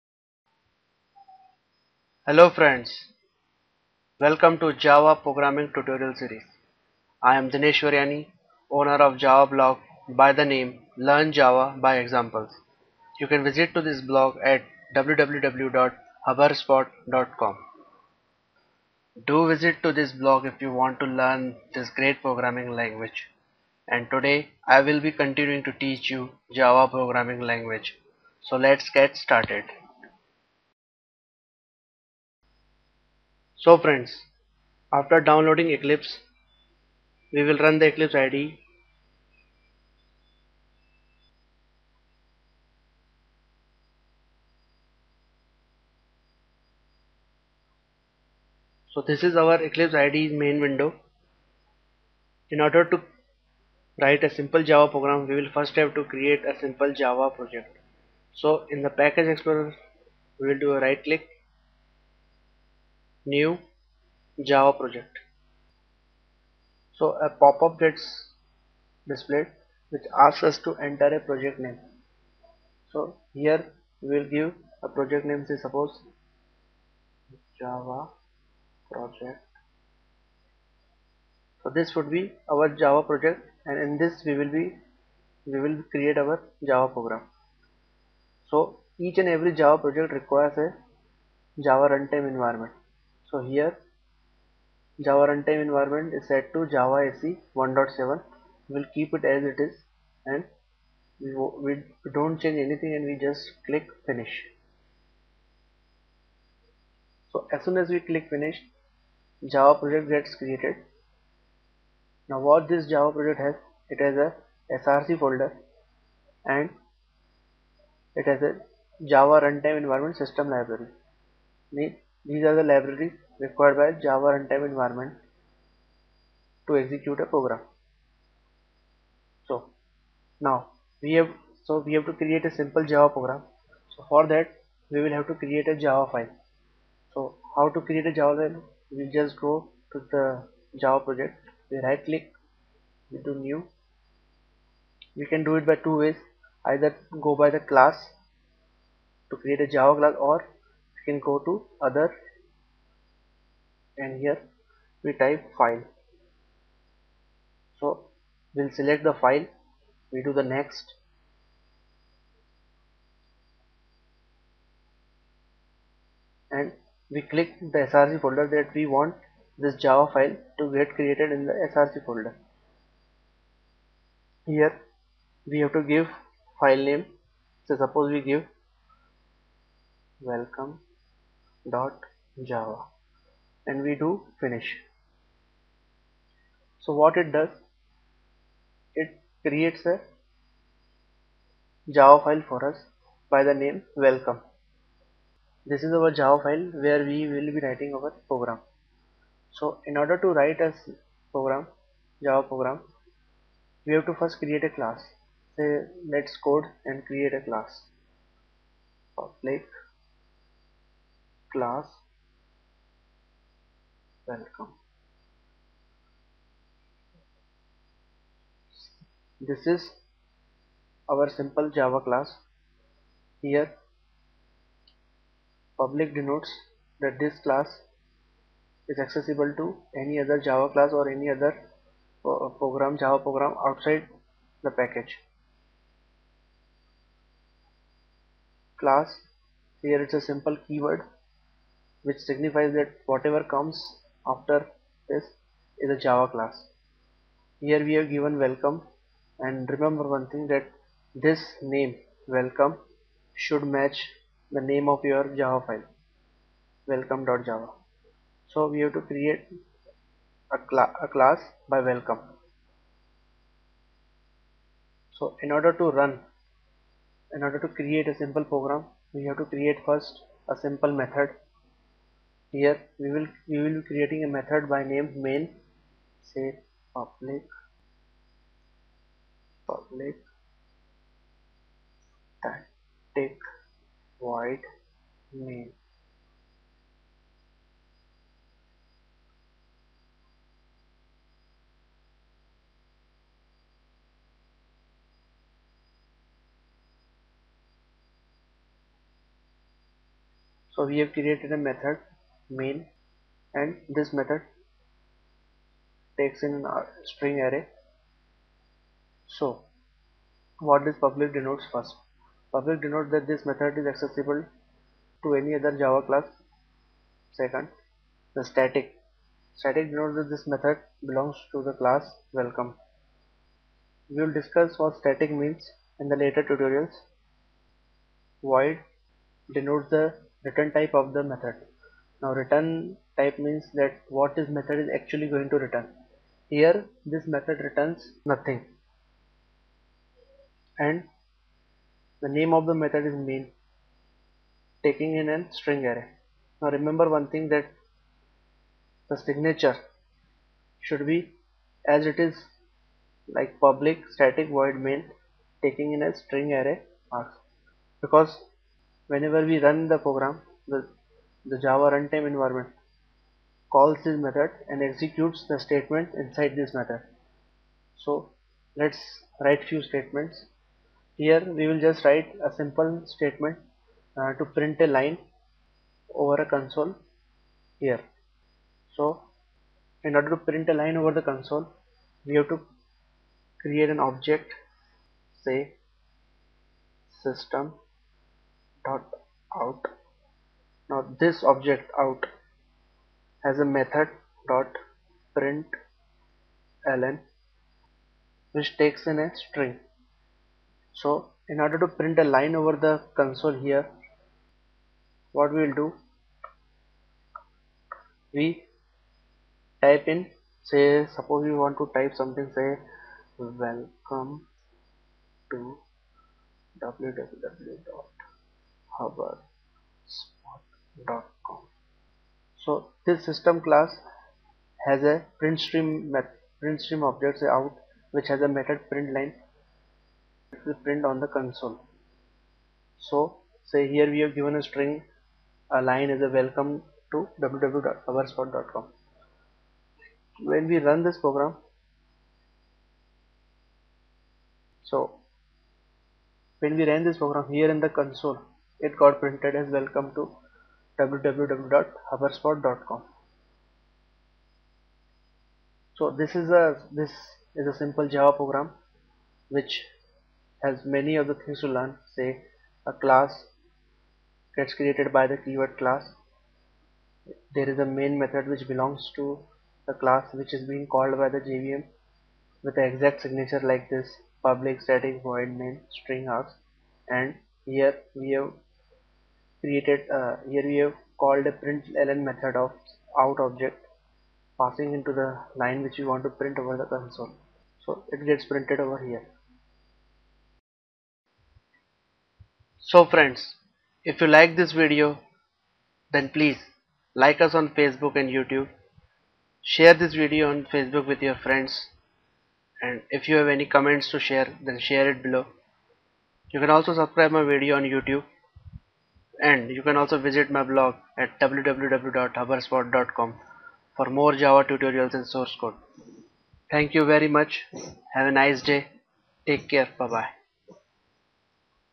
How to create and run a simple Java program in Eclipse IDE ?.
Lets create and run simple Java program in Eclipse IDE. We will learn basics of Java programming language ...